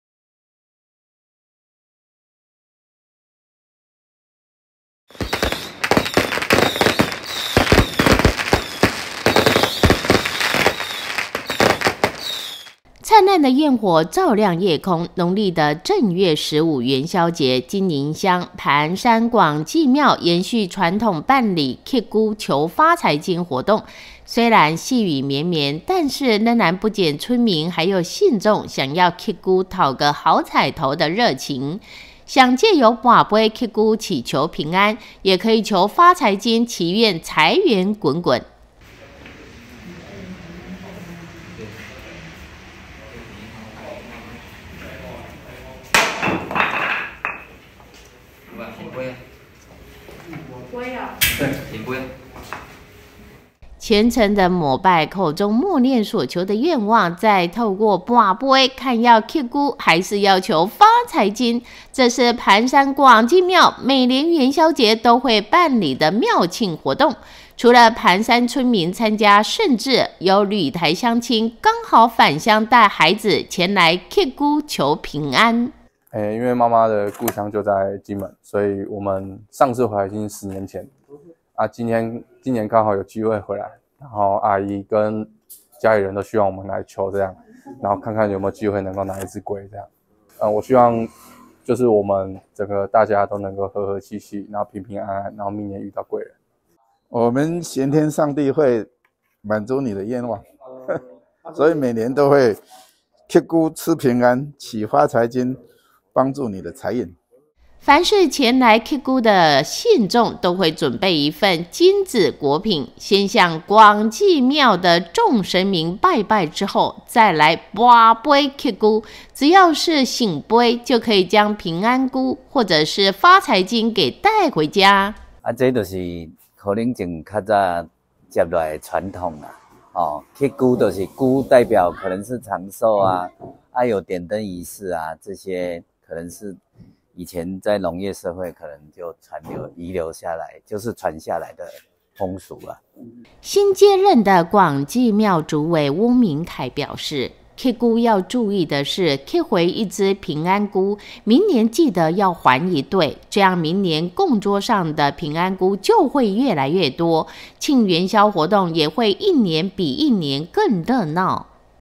名城報報-盤山廣濟廟慶元宵 信眾「乞龜」求發財金
燦爛煙火照亮夜空。農曆正月十五元宵節，金寧鄉盤山廣濟廟延續傳統辦理乞龜求發財金活動，雖然細雨綿綿但仍不減村民及信眾想要「乞龜」討個好采頭的熱情，藉由博杯「乞龜」祈求平安，也可以求發財金祈願財源滾滾。

虔誠膜拜、口中默念所求的願望，再透過博杯看要乞龜還是求發財金，這是盤山廣濟廟每年元宵節都會辦裡的廟慶活動，除了盤山村民參與，甚至有旅台鄉親剛好返鄉帶著孩子前來乞龜求平安。

凡是前來乞龜的信眾，都會準備一份金紙果品，先向廣濟廟眾神明拜拜後，再來博杯乞龜，只要是「聖杯」就可以將平安龜或著是發財金帶回家。

新接任的廣濟廟主委翁明楷表示，乞龜要注意的是乞回一隻平安龜，明年記得還一對，這樣明年供桌上的平安龜就會越來越多，慶元宵活動也會一年比一年更熱鬧。